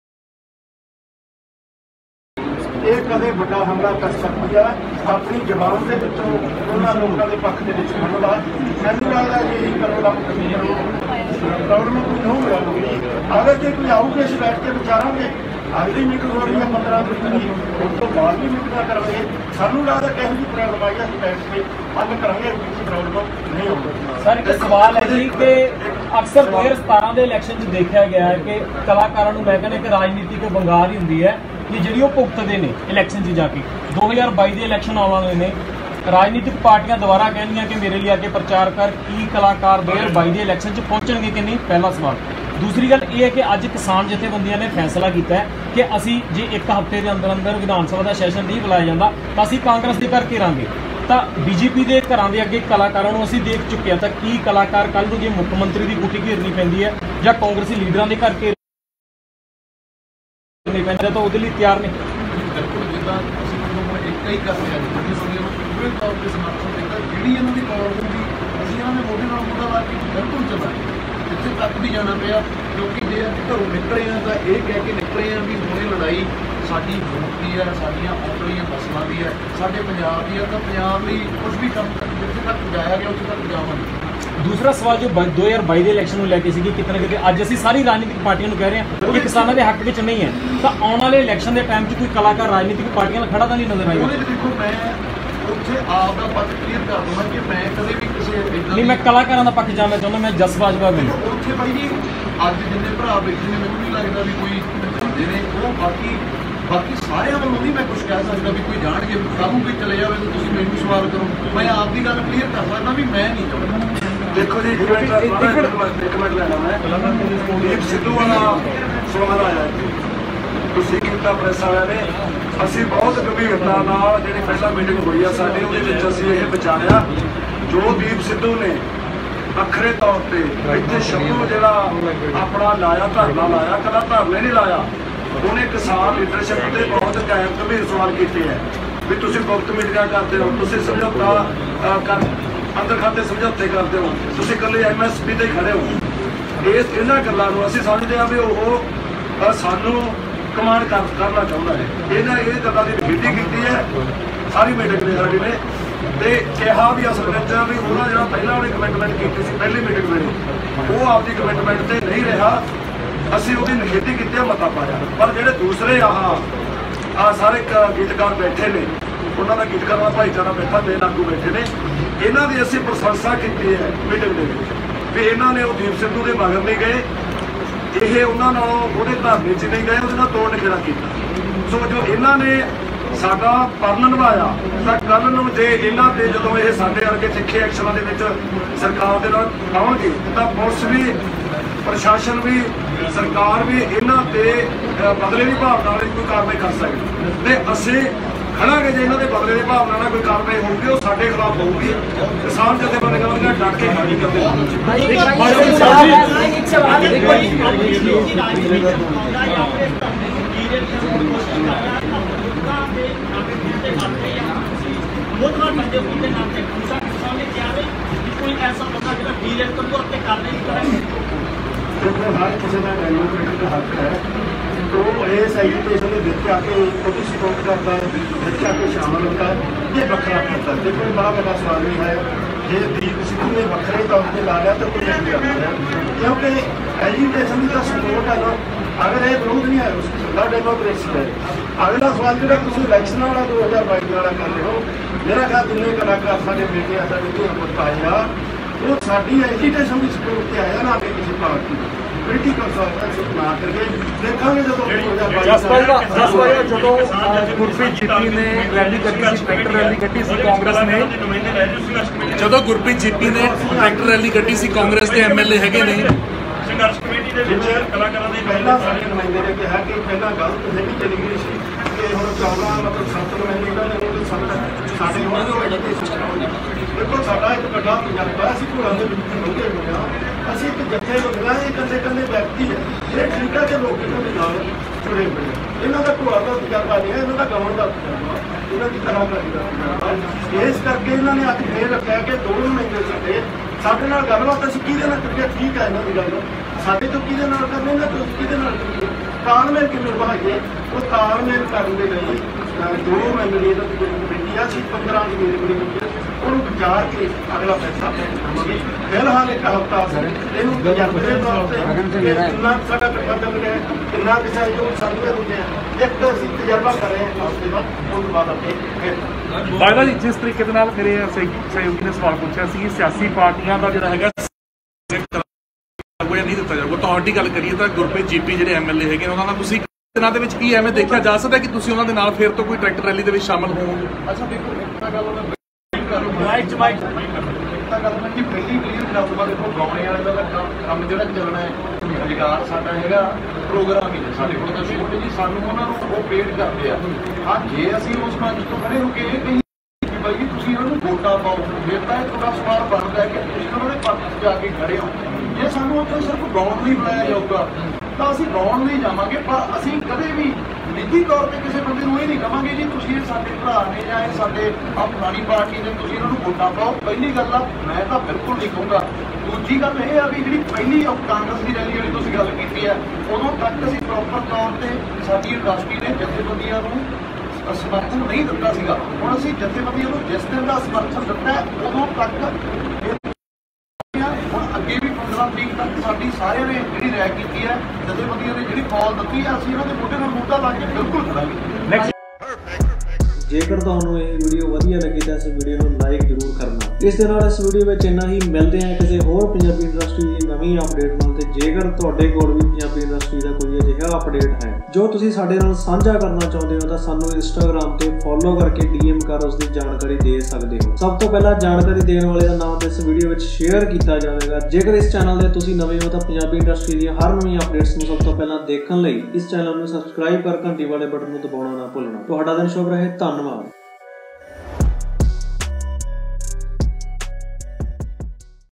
LIVE Press Confrence Chandigarh I Jass Bajwa, Sippy Gill, Kartar Cheema, Harf Cheema I Kissan Ekta
Newsline Team